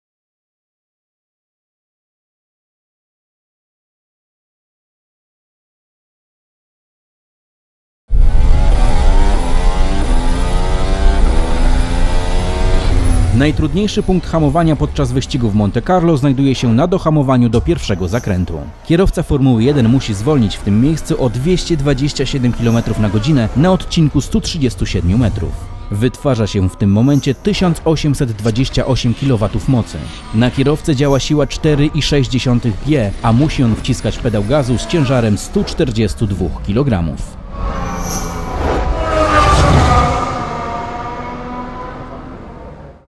Najtrudniejsze miejsce hamowania na torze Monaco
Dohamowanie do pierwszego zakrętu Toru w Monte Carlo jest najtrudniejsze dla hamulców, o czym informuje Brembo